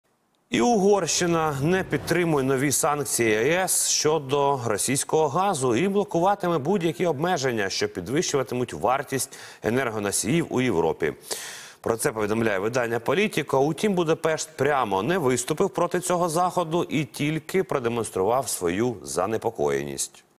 Угорщина не підтримує нові санкції ЄС щодо російського газу
Угорщина не підтримує нові санкції ЄС щодо російського газу - Politico. Угорщина не підтримує нові санкції Європейського Союзу щодо російського газу. Як передає Укрінформ, про це повідомляє Politico з посиланням на дипломатичні джерела.